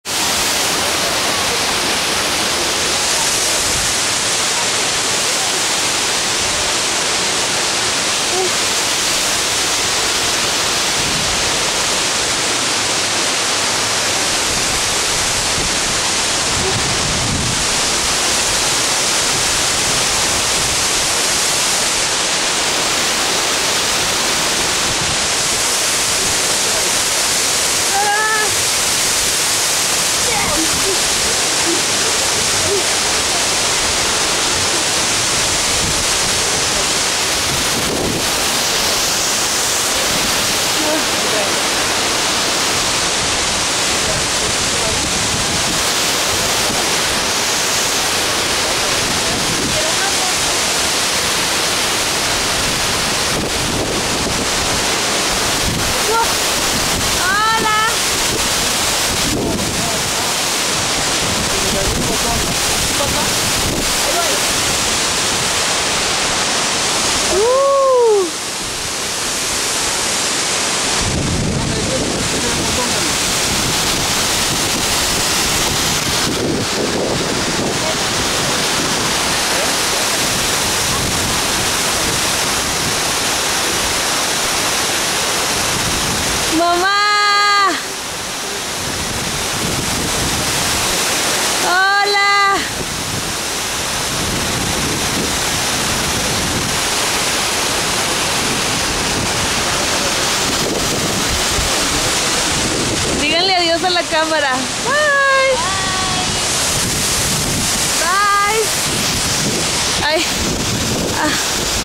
Cascada Cusarare (walking)
Me walking in the lower part of the water fall =)
Cusarare Waterfall
Creel, Chihuahua, Mexico